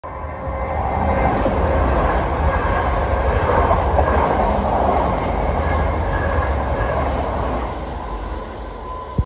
Class 444 + 450 Desiro | 444021 + 450096 | South Western Railway
A pairing of SWR Desiro Units speed through.